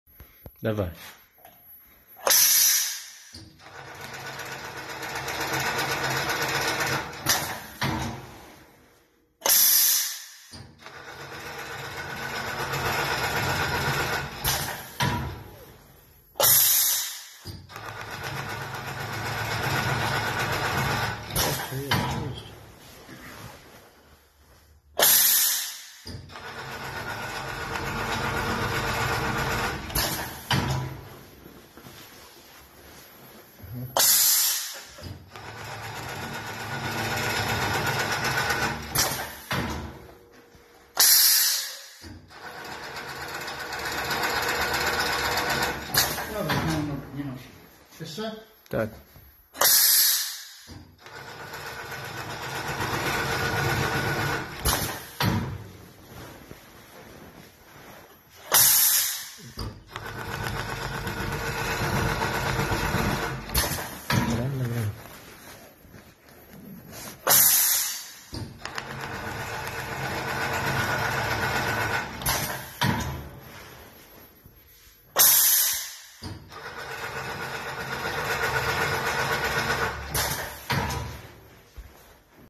Pikowarka HECTOR STYRTEKNIK 600 mm,najlepsza,niezawodna
Pikowarka HECTOR STYRTEKNIK 600 mm,
najlepsza,niezawodna na rynku do pikowania kółkami.poduszki ,materace ogrodowe i legowiska dla zwierząt.
W komplecie stoły i mechanizm do szpulowania.
Podstawowe dane techniczne: długość robocza ramienia 600 mm, wznios stopy 170mm, szycie kresek o maksymalnej długości 30 mm, szycie kółek o średnicy maksymalnej 30mm, zasilanie 230V, pneumatyka min. 6 bara
Tex-Prim Mieczysław Kacprzak
 Maszyny szwalnicze
Łagiewnicka 272 A
91-509 Łódź